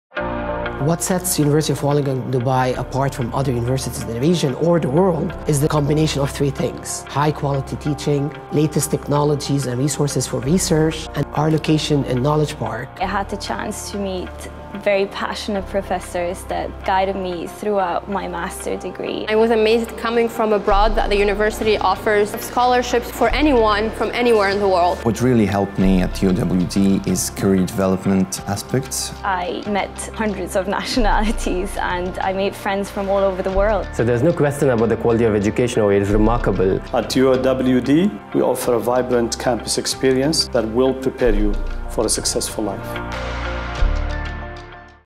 Why choose UOWD?
Hear from our Alumni, current students and faculties on why UOWD is the best choice for you.
Want to know more about our programs, intake dates, scholarships etc?